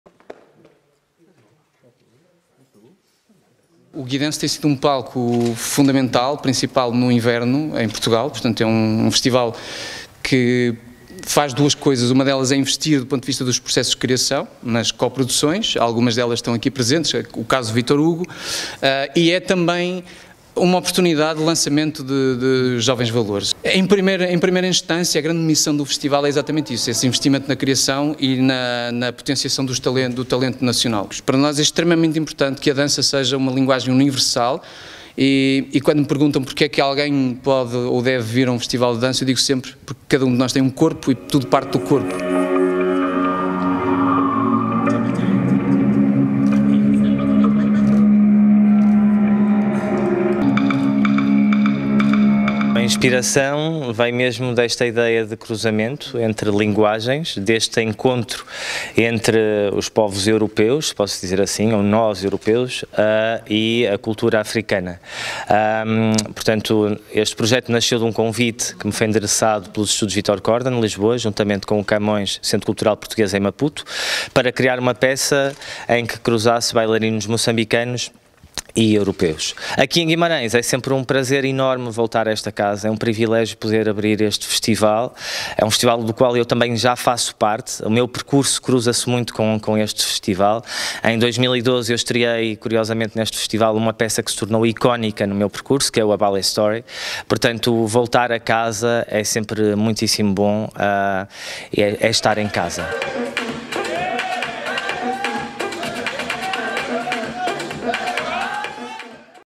Guimarães vê a "Humanidade a dançar" na 13.ª edição do GUIdance
O GUIdance dá esta quinta-feira início à sua 13.ª edição, num evento que pretende pôr a “Humanidade a dançar em Guimarães”, mostrando em palco o que distingue as pessoas e como a dança as pode unir. A abrir vai estar 'Bantu', o mais recente trabalho do coreógrafo vimaranense Victor Hugo Pontes, que junta intérpretes europeus e africanos, que criam em palco uma linguagem comum.